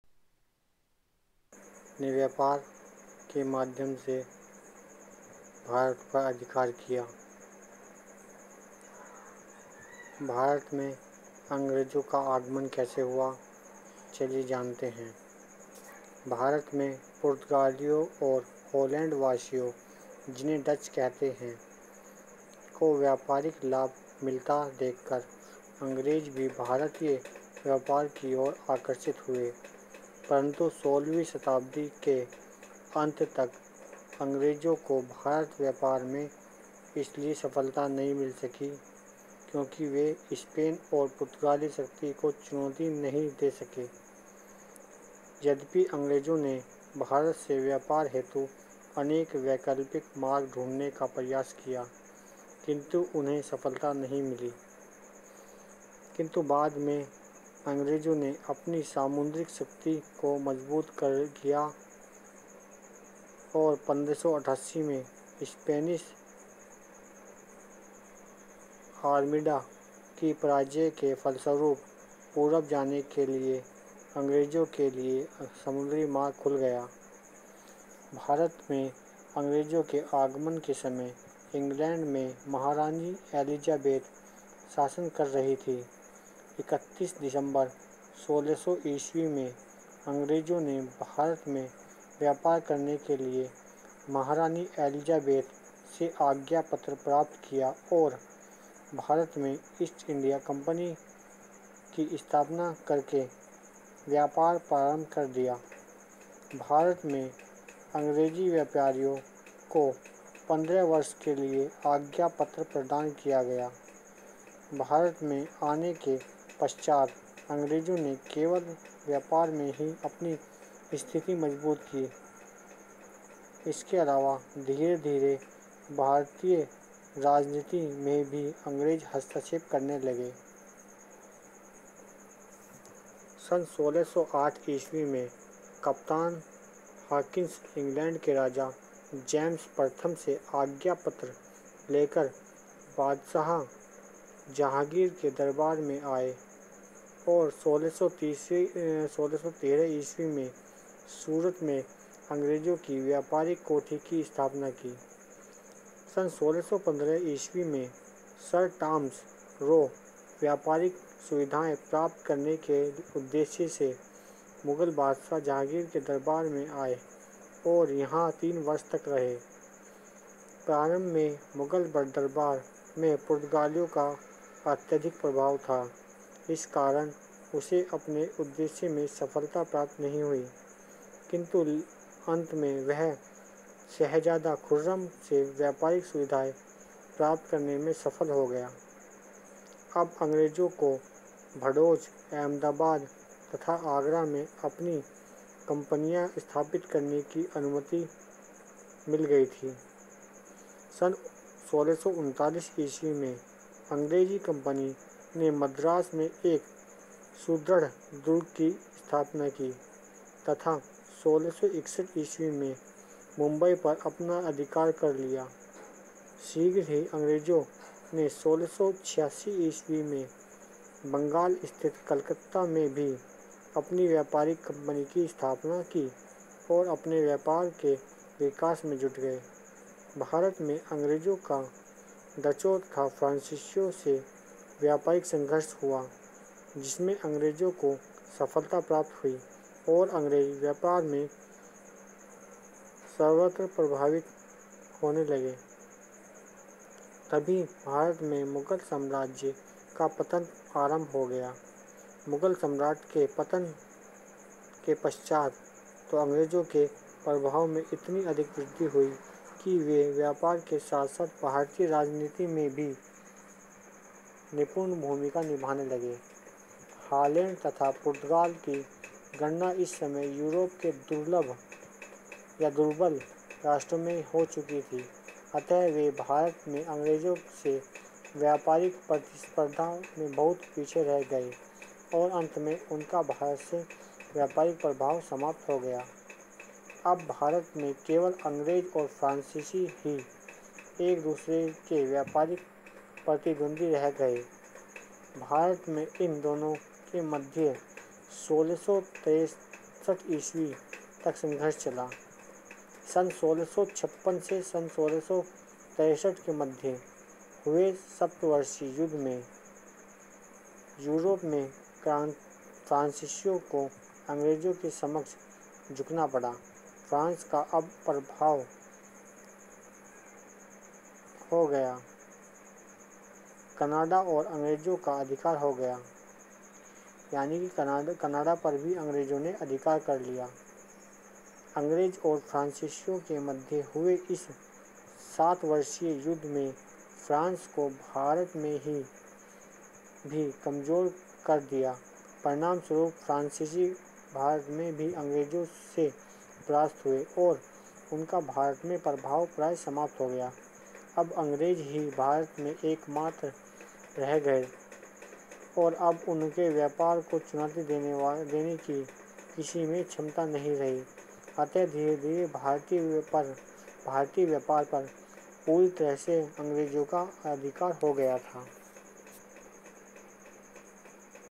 History of the British Raj - अंग्रेज भारत क्यों आये - British East India Company
Why British comes in india, full history of british raj in india, and hisoty of British East India Company in india, and you will know what is british east india company, reasons of British raj,